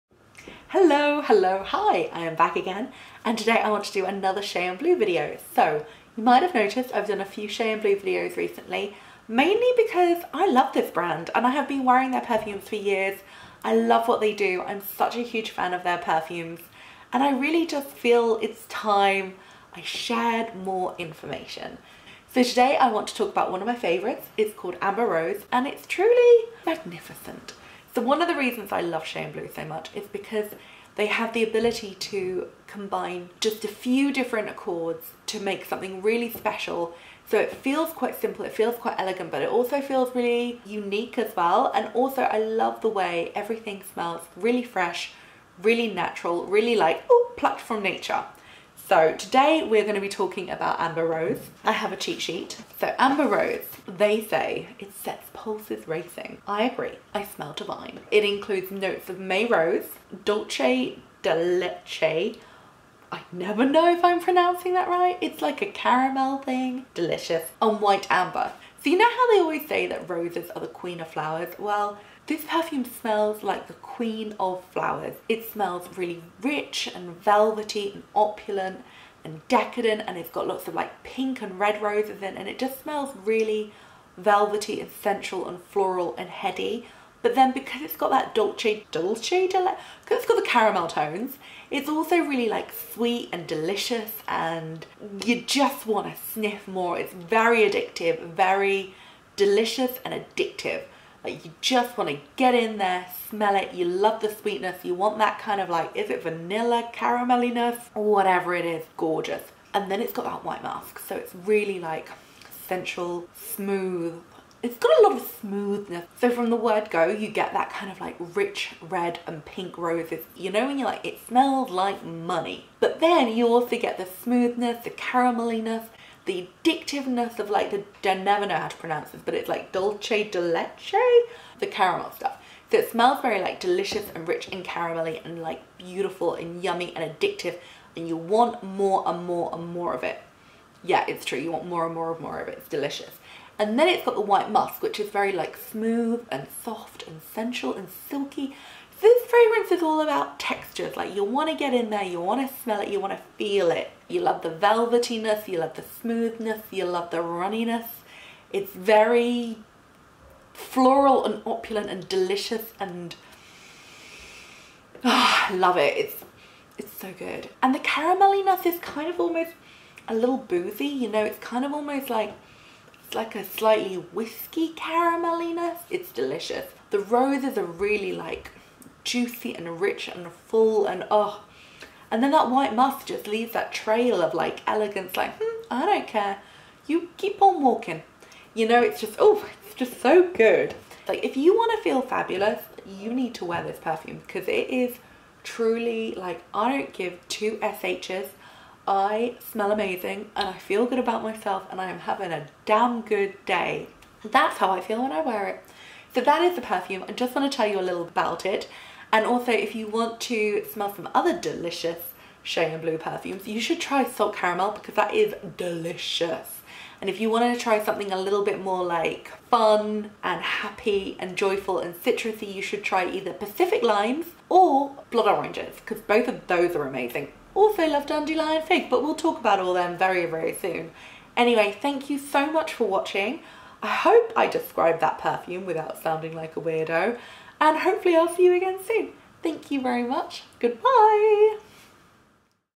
Shay & Blue 'Amber Rose' Perfume Review! OMG!! Roses & Caramel!!!
A decadent, elegantly romantic fragrance. Amber Rose marries rare May Rose from Grasse with a voluptuous sweet milk note resting on a base of white amber to create a chic and modern rose scent.

Top Note - A soft opening melody of precious, rare May Rose from Grasse for an immaculately feminine first impression.
Heart Note - At its heart is a symphony of delicious dulce de leche with sweet milk tones that are voluptuous and enveloping.
Base Note - Approaching the beauty of sundown fine white amber radiates the softness of a warm dusk.
In a rich, long-lasting Eau De Parfum concentration.
Beautifully wrapped in a gift presentation box with striped grosgrain ribbon.
Handcrafted in England with real flowers, fruits and spices. This perfume contains around three pounds of roses. Amber Rose was created in 2012 by Julie Massé, Perfumer, and Dom De Vetta, Founder. We do not test on animals. Find us on the PETA list of cruelty-free companies.

xxxxxxx